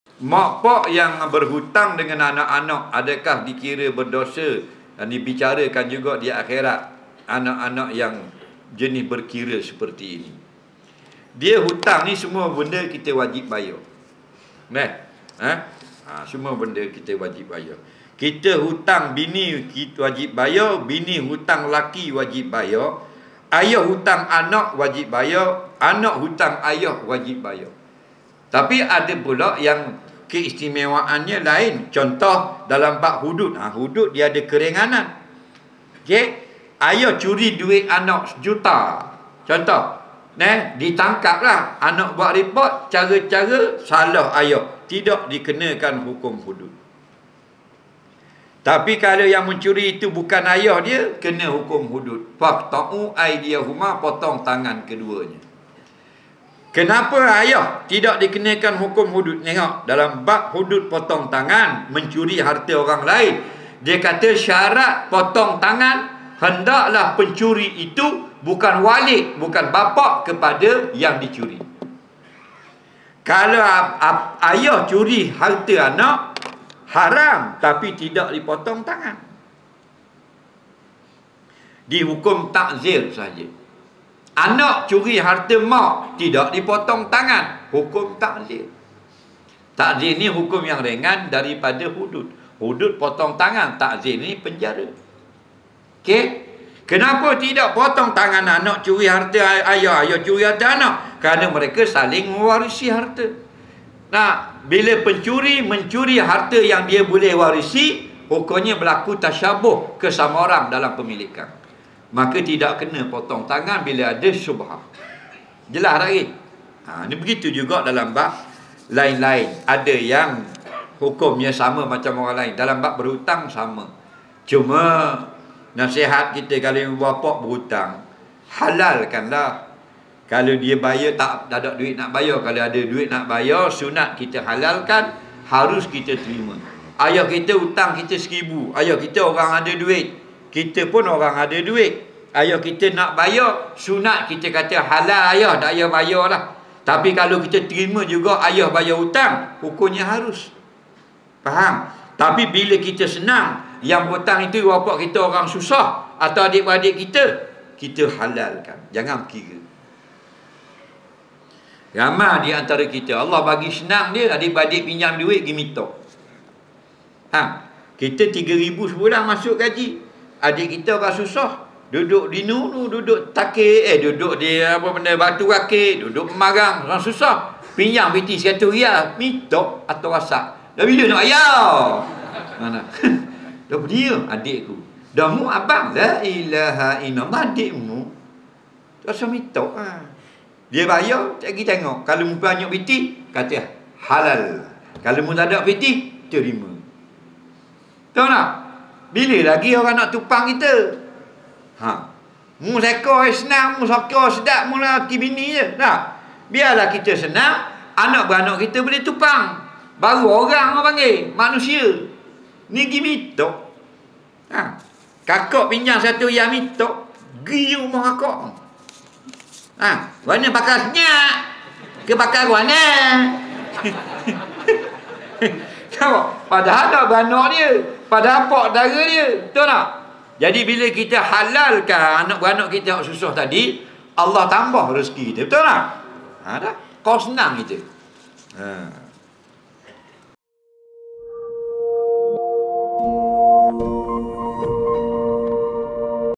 Mak Ayah Berhutang Dengan Anak Disoal Di Akhirat - Ustaz Azhar Idrus
📆 08 September 2023 | Jumaat
⏰ 07.45 malam
🕌 Surau Al-Hijrah, Hulu Takir, Kuala Nerus, Terengganu
📖 Kitab : Anak Kunci Syurga